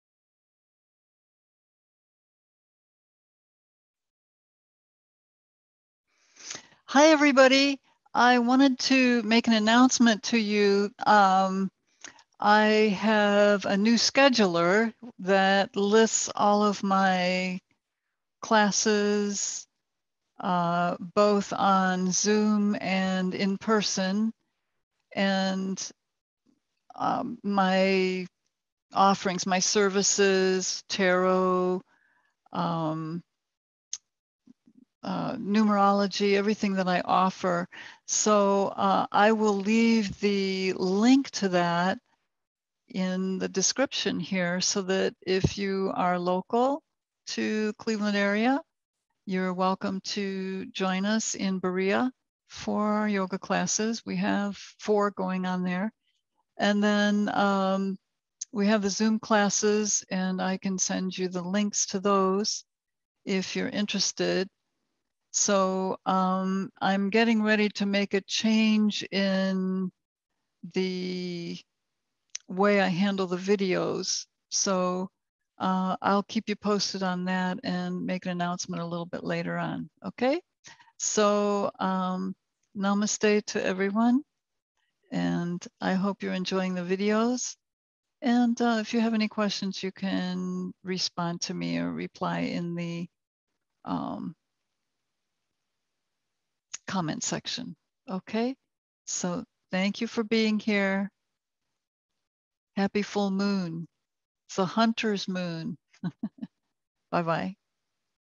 Michelle Star Yoga - Announcement 10-20-21
New Scheduler (it's pretty!) - Now you can order and purchase classes, Tarot/Angel Card readings, Reiki (in person only), Numerology reports (soul path and Prosperity Calendar for the rest of 2021), online, including Zoom or in person if you are local. Check it out! Link: